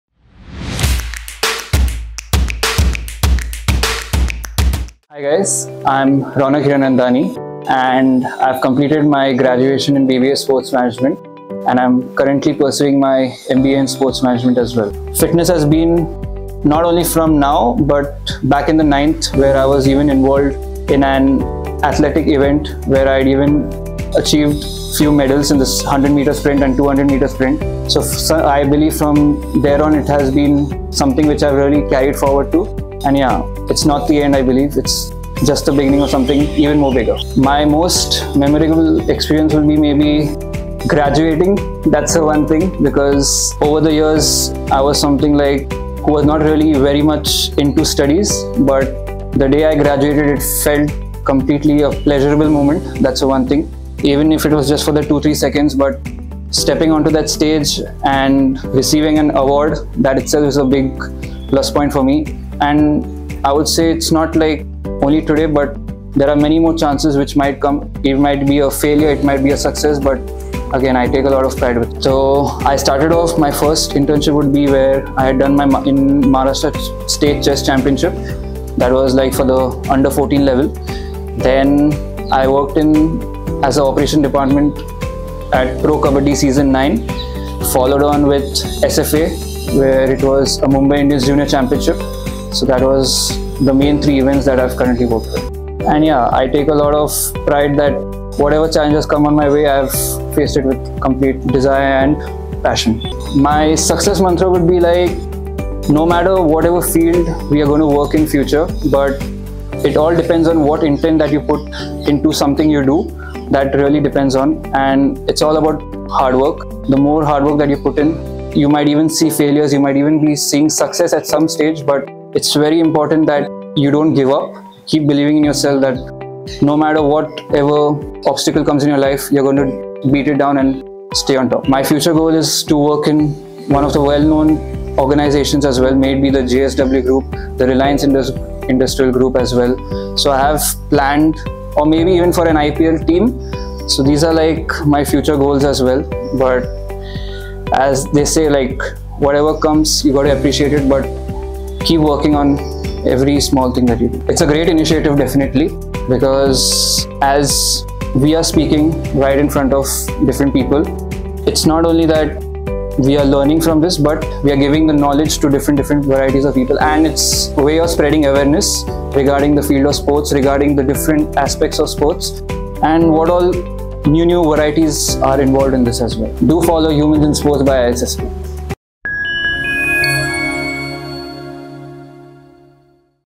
Human 09: Ronak Hiranandani | Sports Management Professional
Human 09: Ronak Hiranandani
Our next human is from Kuwait "Ronak Hiranandani"a Sports Management Professional who has worked at various elite sporting events and leagues. And here is his story.

Follow @humansinsportsbyisst for such amazing and inspirational stories.